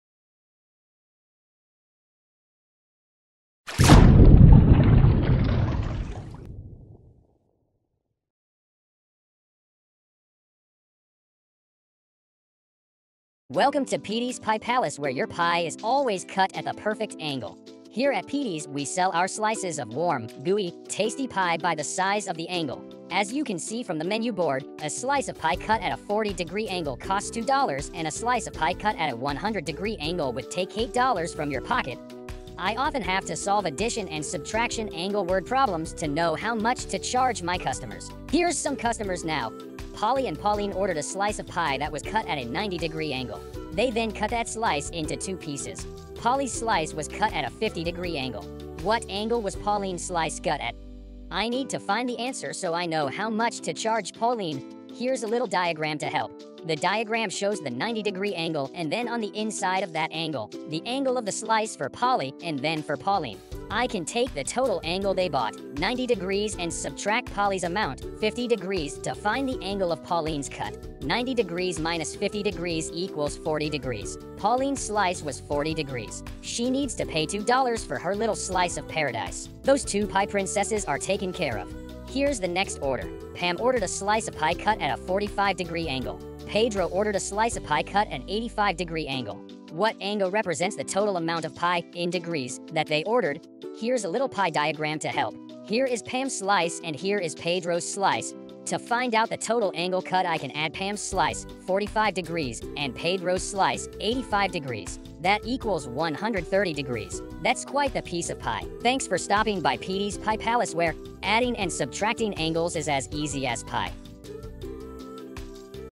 Add and Subtract Angles Word Problems | 4th Grade Math
Underwater Math provides engaging learning solutions for students. This video helps students solve word problems adding and subtracting angle measurements. Alabama Standard M.24, M.25, M.26